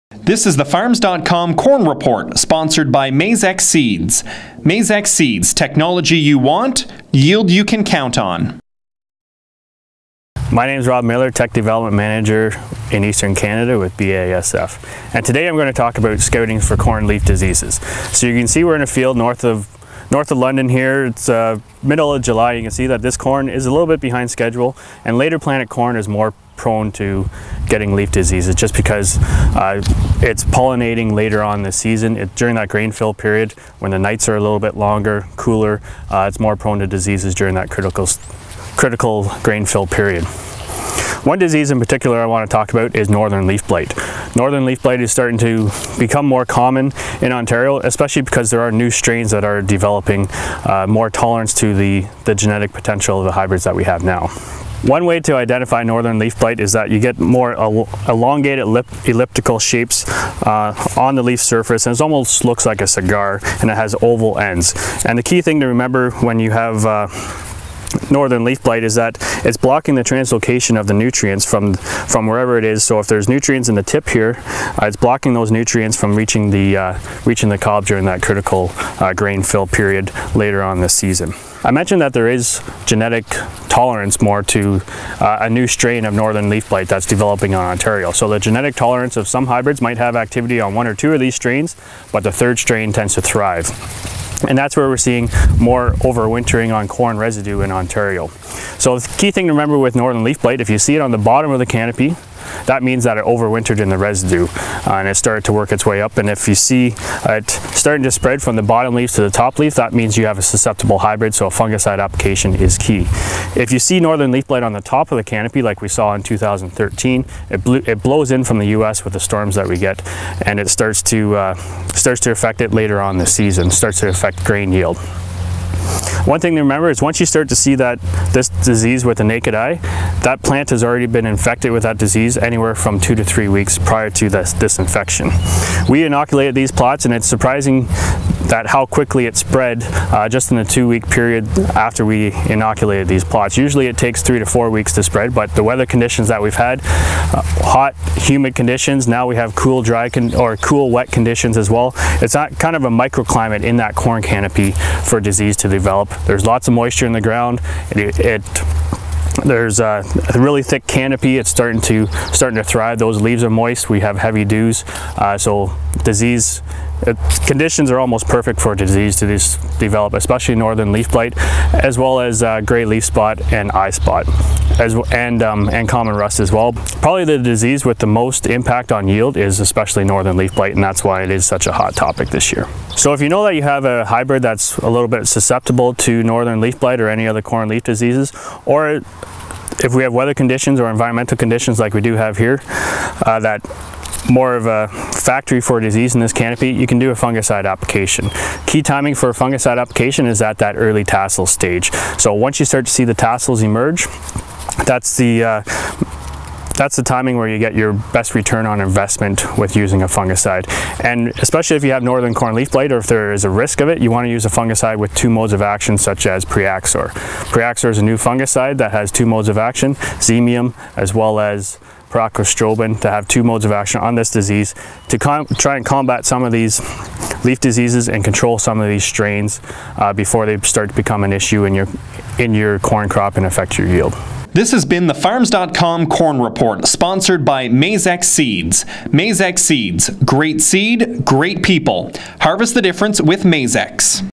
Farms.com Corn Report: Scouting For Corn Leaf Disease.
Farms.com Corn Report: Rob Miller, Technical Services Manager with BASF Canada provides some tips on how to scout for corn leaf diseases.  He also describes how to identify Northern Leaf Blight and what farmers can do to manage for highest yield potential.